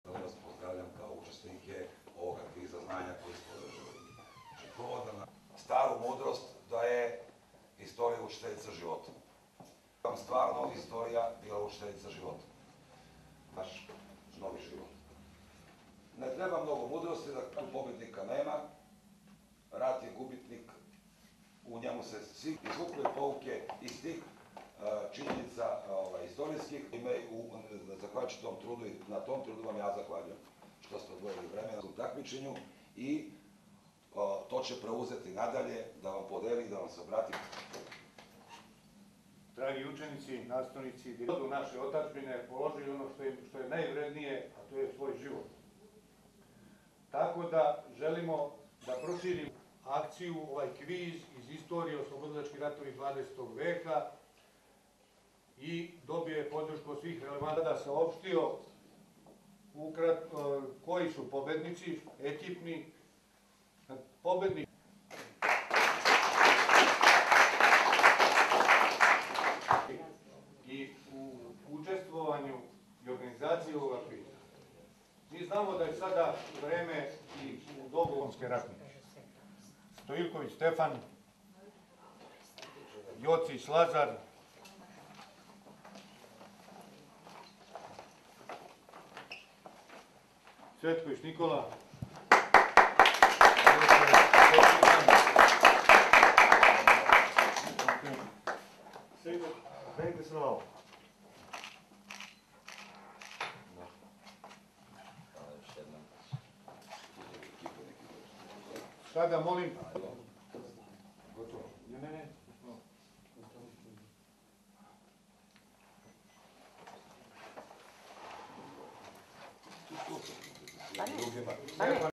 Nis, opstina Pantelej, Dodela medalja i diploma osnovcima, pokrivanje.m2p
Savez potomaka ratnika Srbije u Nišu dodelio je medalje, diplome i druga priznanja najbolje plasiranim učenicima i školama na području opštine Pantelej. Osnovci su pokazali odlično znanje na nedavno održanom takmičenju iz poznavanja istorije Balkanskih i srpskih odbrambenih ratova, a prva tri mesta osvojili su takmičari iz osnovnih škola: Čegar, Stefan Nemanja i Njegoš.